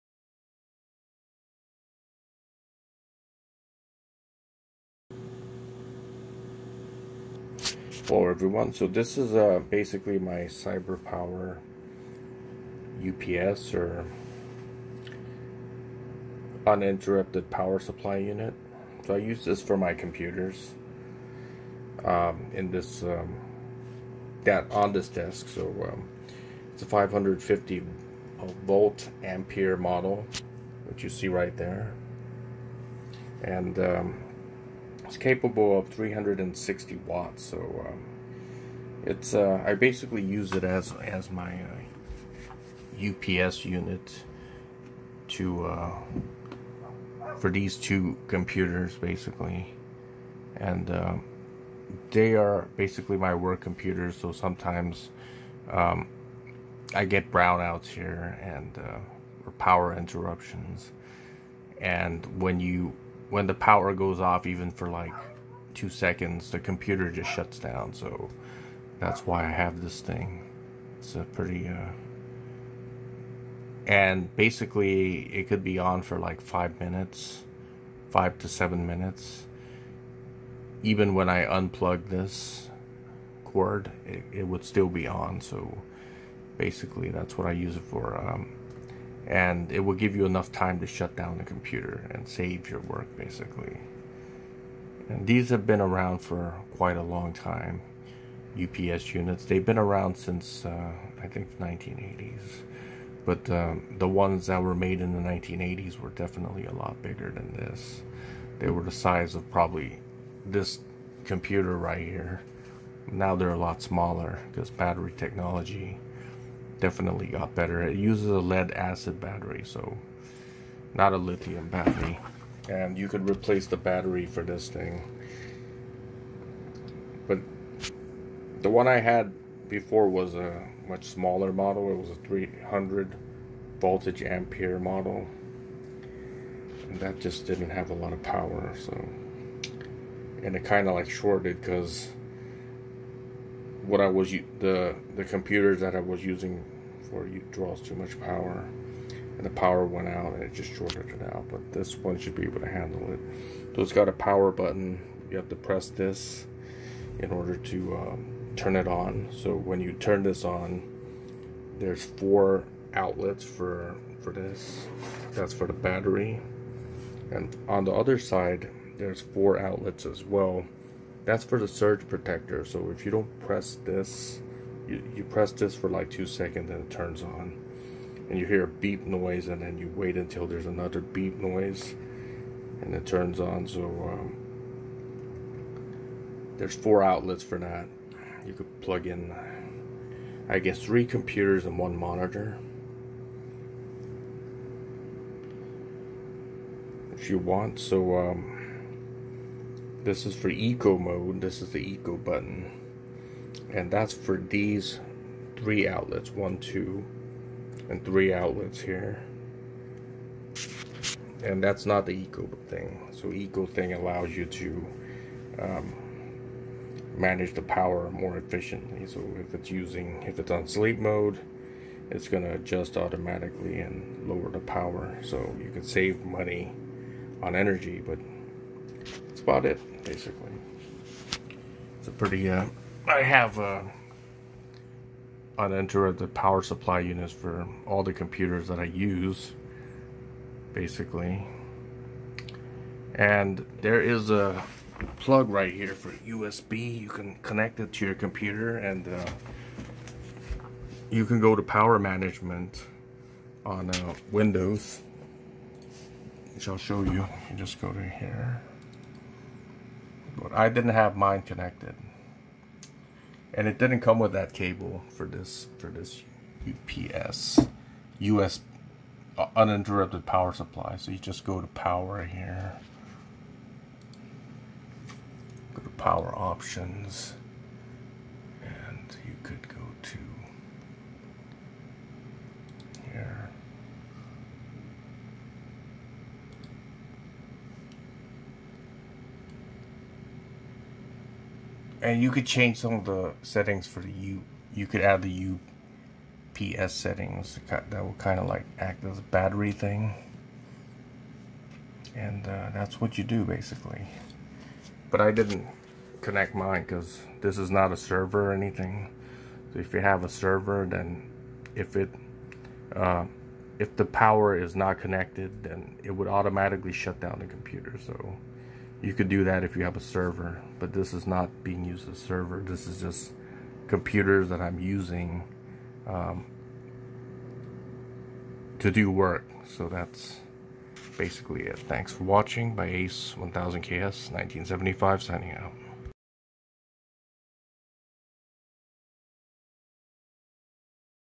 CyberPower EC550G UPS
[Synopsis]

This video is a review video about the CyberPower EC550G UPS(Uninterrupted Power Supply) unit. Devices like these are battery backups for desktop computers, so when the power goes out desktop computers don't have batteries, like laptop computers. As a result, the desktop computer just shuts down abruptly even if the power was cut for 2 seconds. This can be bad if you are doing work, because you can lose your work when this happens. You could have been working on something for 2 hours, and lose that work if a brownout or a blackout happens. Also shutting down your computer abruptly like this can corrupt the files in the operating system, and this can cause problems in the future.

The way UPS units are rated is in voltage amperes, so this UPS has 550 VA on it. 550 VA does not equal to 550 Watt. VA means units of measurement of electrical power. Watts on the other hand implies real power. VA = Vdc x Idc . Vdc means voltage in direct current circuit, and Idc means current in a direct current circuit. In a DC circuit VA does equal Watts. When this is converted to AC circuits, it is much less, like 60% to 70%. As a result, a 550 VA UPS is rated at 550 VA x  (0.60) =330 Watts .

You do have to replace the lead acid battery in this thing every 2 to 3 years, but it would more than pay for itself. Losing hundreds of hours of productivity is the alternative, so I would say it would be worth it. If you are updating your BIOS, and if a brownout or blackout happened that would be very bad. A UPS gives you time to shutdown your computer, and you can also install software from CyberPower to have the computer shutdown the computer automatically if power is cut off. A UPS is a must for those who use servers, because servers can shut themselves down automatically if power is interrupted.
[Links]

♦ CyberPower EC550G Ecologic Battery Backup & Surge Protector UPS System, 550VA/330W